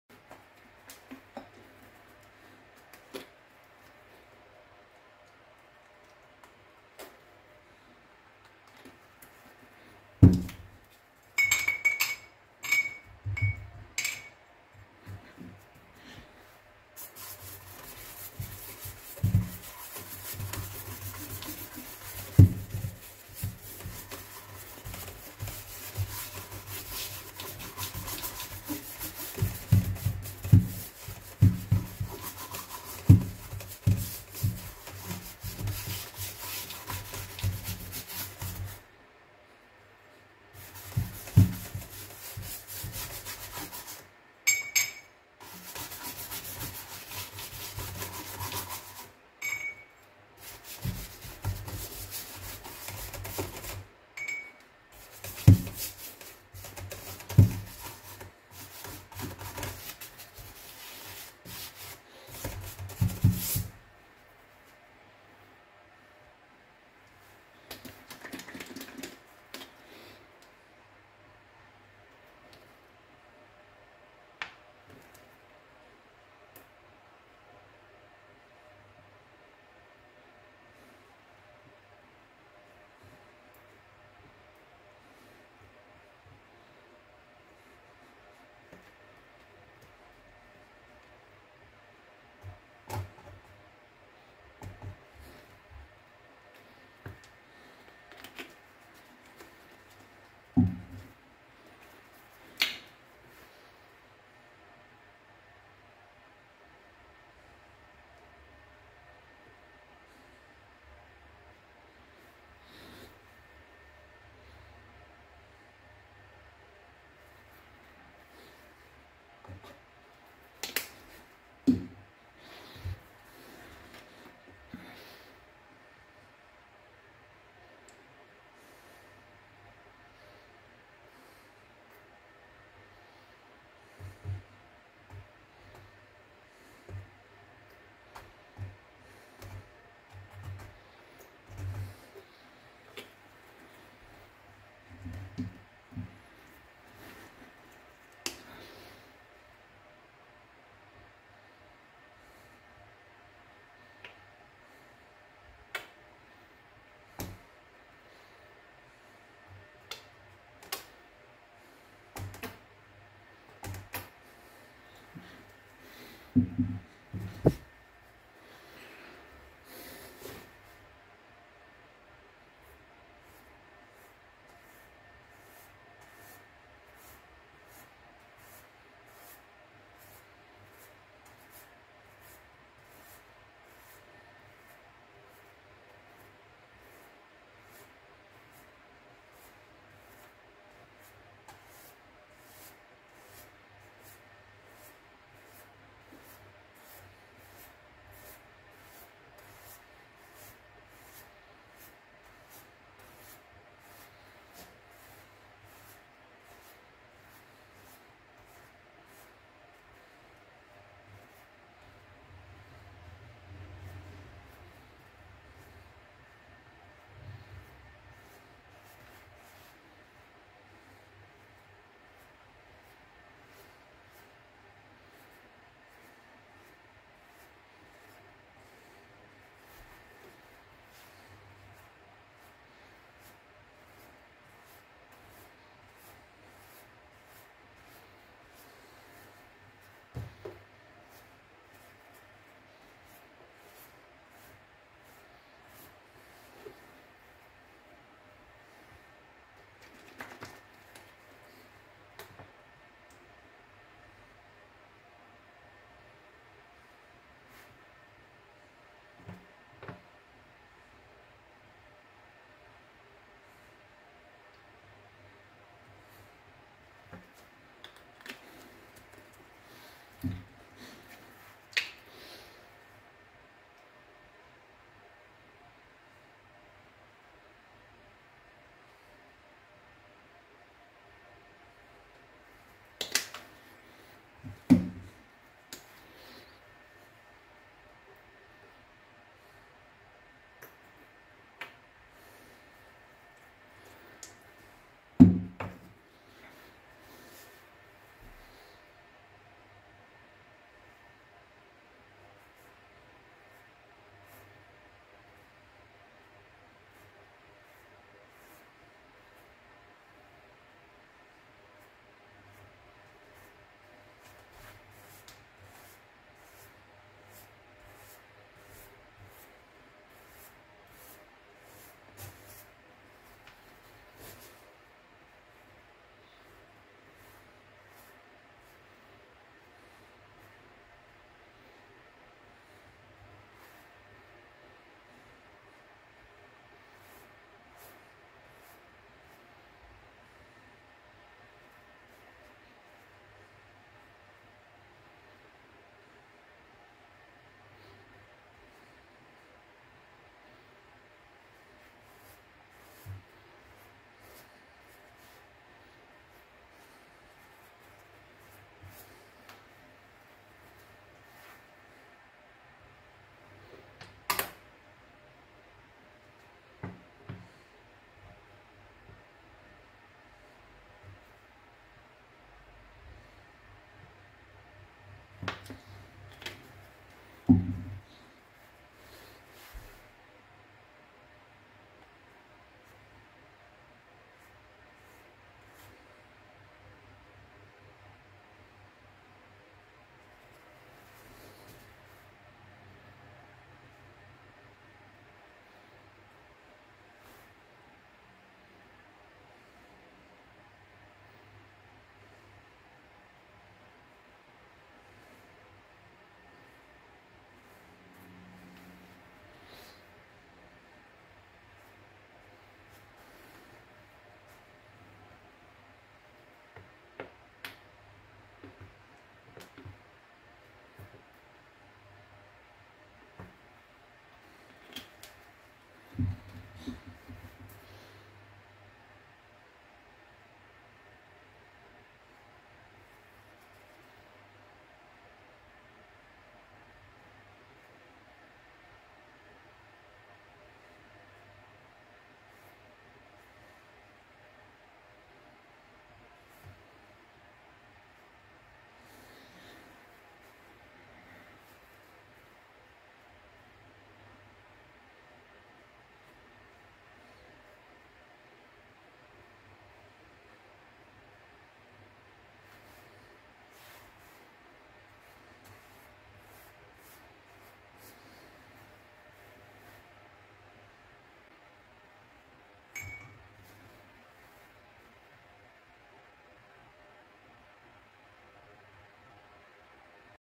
Mare Nero / Dipinto Acrilico Astratto
dipinto astratto ad acrilico con 4 colori: bianco, nero, blu e azzurro.

fatto su un cartone riciclato grande 45 x 35 cm.

Buona visione